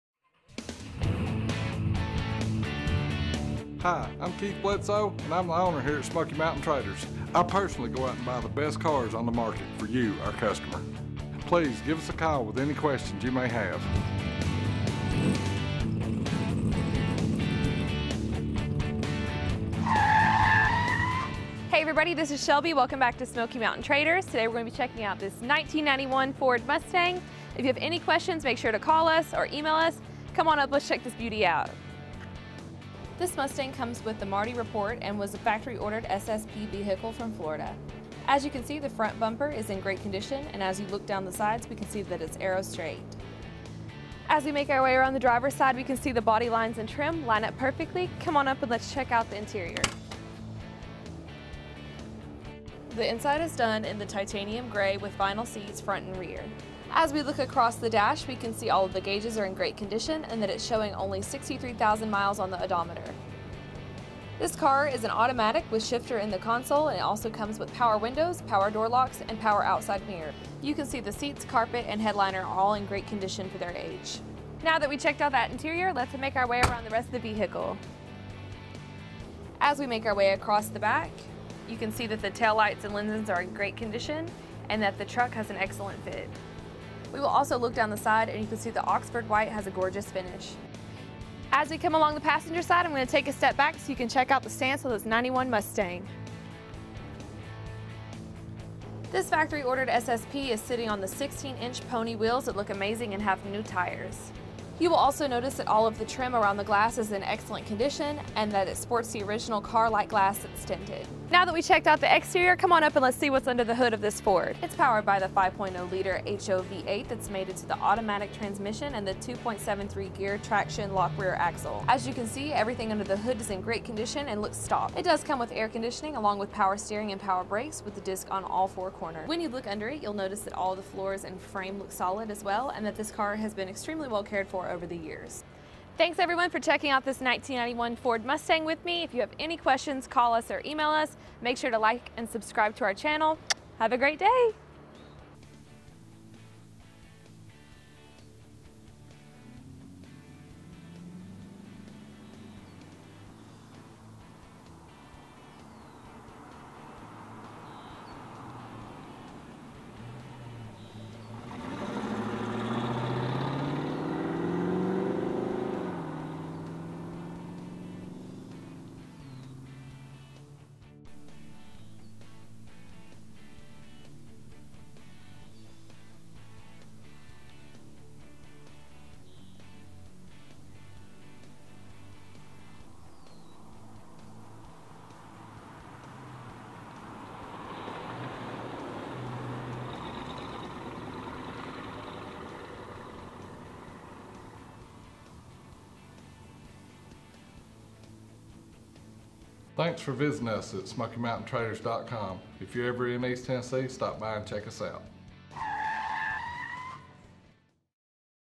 1991 Ford Mustang LX SSP For Sale
Here we have a 1991 Ford Mustang LX SSP for sale. This car has a clean CarFax with no accident history. It comes with a Marti Report and was factory ordered as an SSP vehicle from the Jacksonville Ordering District. We will start off with the exterior of this Mustang. When you walk around it you can see that the body is in excellent condition. This is the LX body style and it is covered in the Oxford White paint that gorgeous shine. All the trim is also in great condition along with all of the glass and seals. This 5.0 has the rear exiting tailpipes that give it a killer look. To finish off the exterior it is sitting on a factory-style Pony Wheels with New Tires.
Next, we will look at the interior. On the inside, it's done in the Titanium Gray with Vinyl Seats front and rear. The seats are gin great condition along with the door panels that House the Power Windows and Power Door Locks. This car also has power outside mirrors and the Automatic Option with the shifter in the console. Looking at the dash you can see that it is in excellent condition with little wear showing. All the gauges are working as they should, and it has 67K miles showing on the odometer which we believe to be original. The headliner has a great fit and the dome light is working. All the carpet looks to be original and in great condition for its age. In the trunk, all looks solid and has the factory carpet with spare and jack.
Finally, we will look at the driveline of this Ford. Under the hood, it is powered by the 5.0L SEFI HO V8 engine that is mated to the Automatic Transmission and 2.73 Geared Traction-Lok Rear Axle. You will notice that it all looks correct under the hood and is in great condition. All the firewall and inner fenders look solid. This car does have Air Conditioning along with Power Steering and Power Brakes with Four Wheel Disc. When you look under this Mustang it is all solid and in great condition. This car has been very well cared for over the years and has plenty of power on the Highway. It handles with ease and the brakes are very responsive and stop with ease.
If we can help you in any way with questions or a specific picture, you can call our office at (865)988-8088.